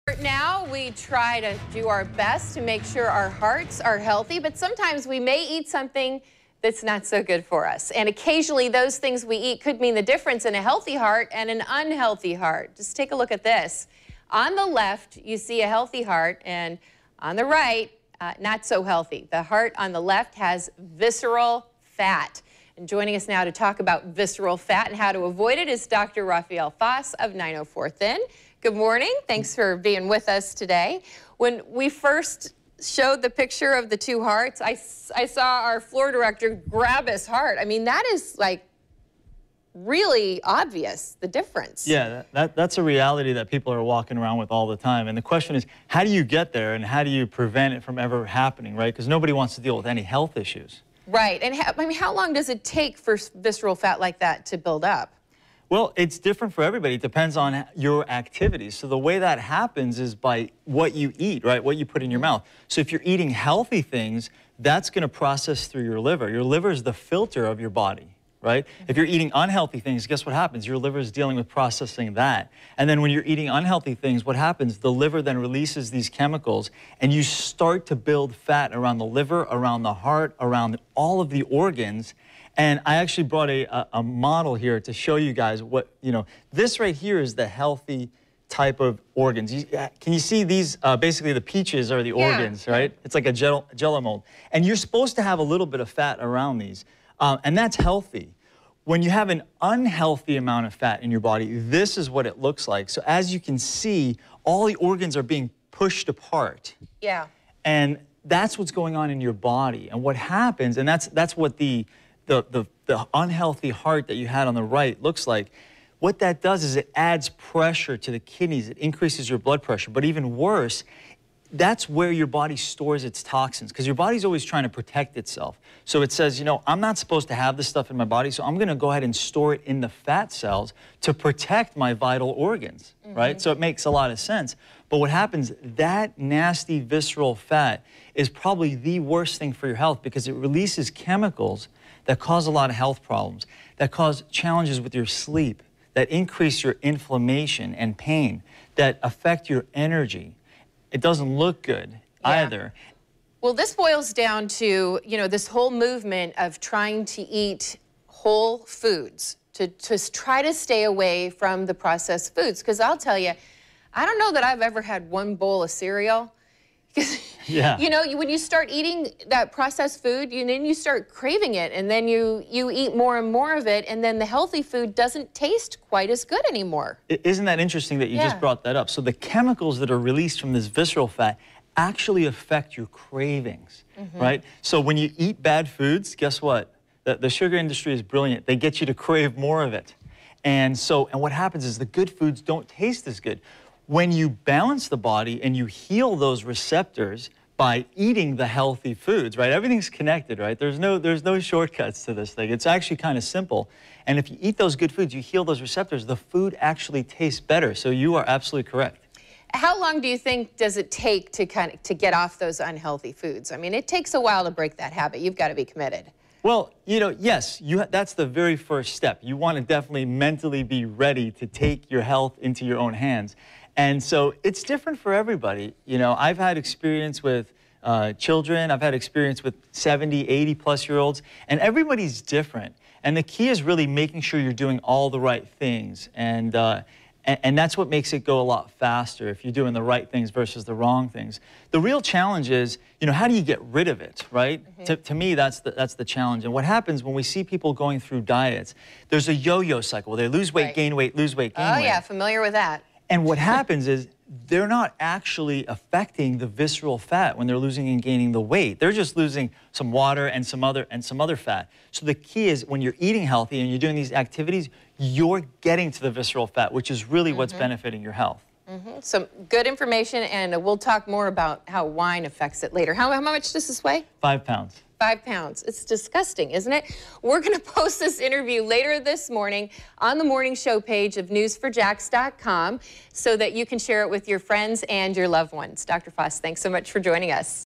Avoiding visceral fat
Dr. Rafael Foss of 904thin explains how to avoid visceral fat.